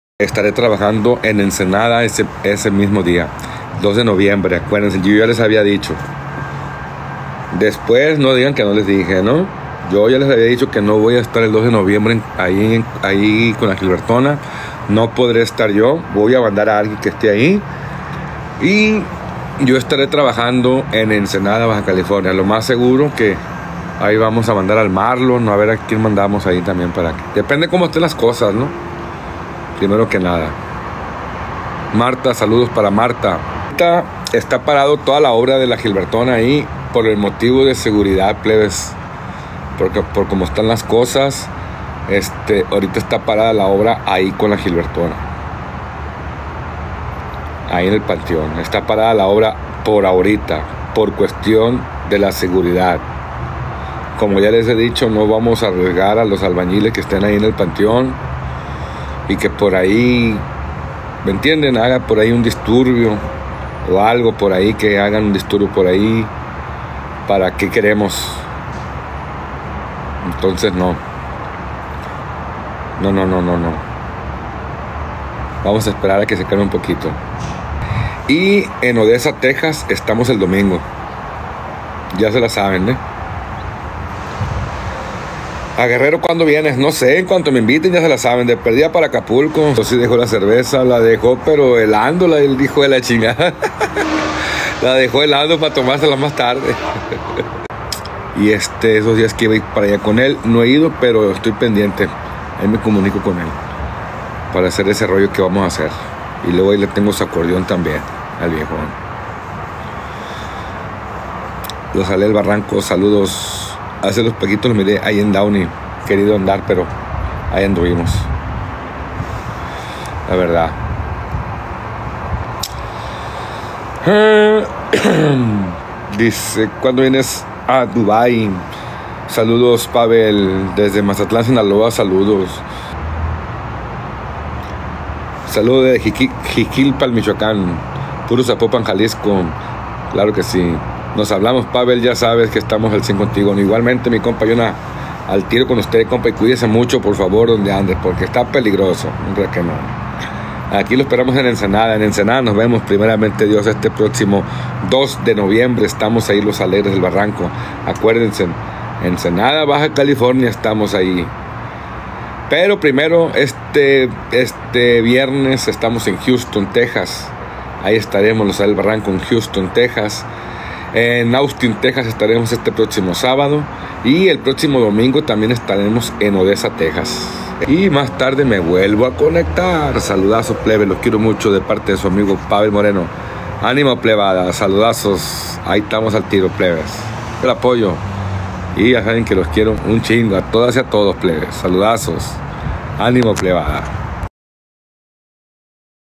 🚨EL 2 DE NOVIEMBRE NO ESTARE CON GILBERTO "MUCHA INSEGURIDAD"MORSER82 😱
El 2 de noviembre no estaré en la tumba de la gilbertona.

SIGUEME PORFAVOR.🫰🥰